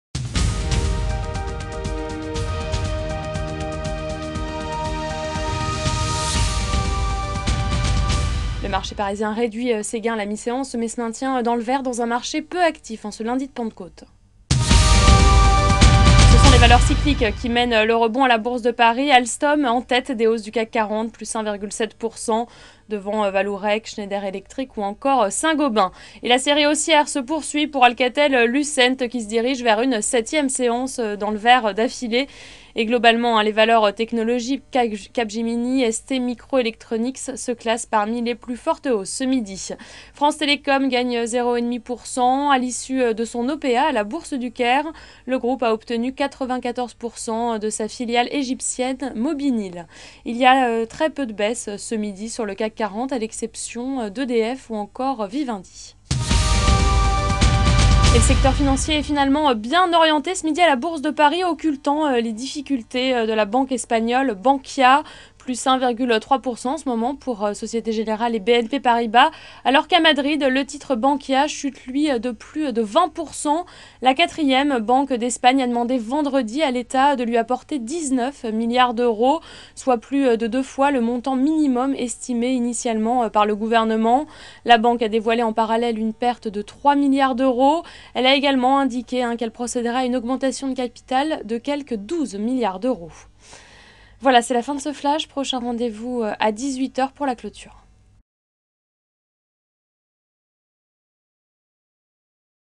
28/05/2012 : Mi-séance : le marché reste bien orienté
Le marché parisien se maintient dans le vert dans une activité réduite en ce lundi de Pentecôte. Les valeurs cycliques et technologiques mènent le rebond...